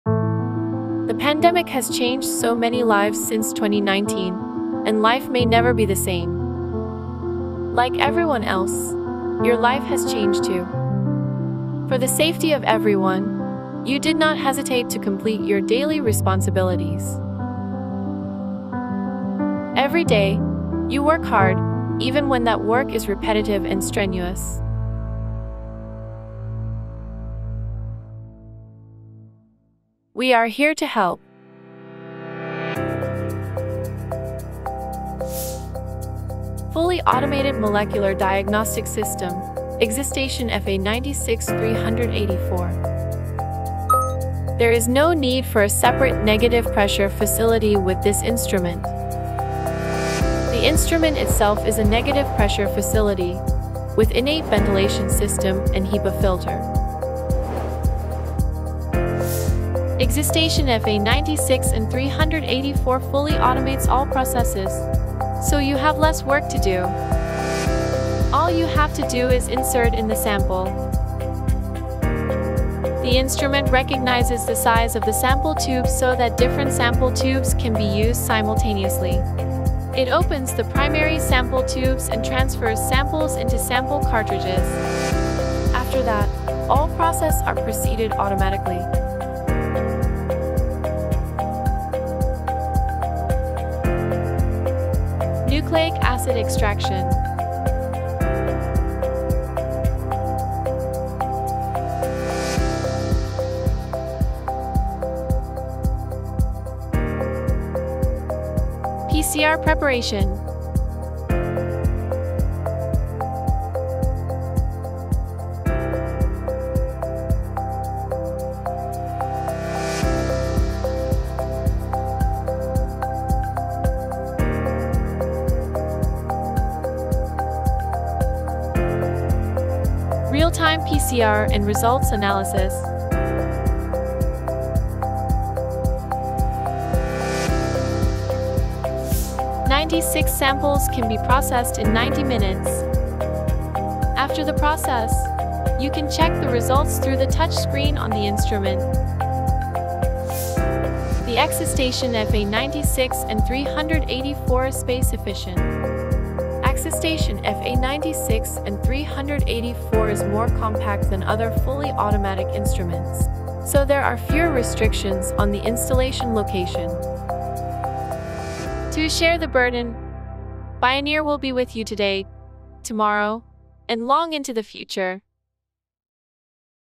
Hands-free! after primary tube loading, ExiStation™ FA 96/384
ExiStation™ FA 96/384, a fully automated MDx system, is no need for a separate negative-pressure facility. All you have to do is insert primary samples. Automated de-capping system for primary tubes, which eliminates the risk of cross-contamination. After that, all processes proceed automatically. After the process, you can check the results through the touch screen on the instrument.